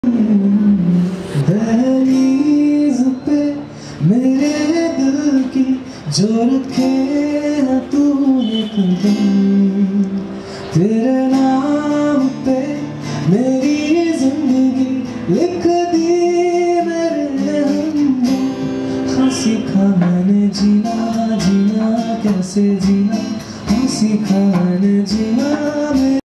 Live Street Music at Nukkad Cafe Pune - December 13, 2015
Artist: Yash, Kedar and Vishal

Come, Listen, Sing, Clap, Tap and Enjoy the Evening.

Live Street Music every Saturday and Sunday at Nukkad Cafe, Pune from 7 pm to 9 pm. There is NO CHARGE for listening music and enjoying the evening.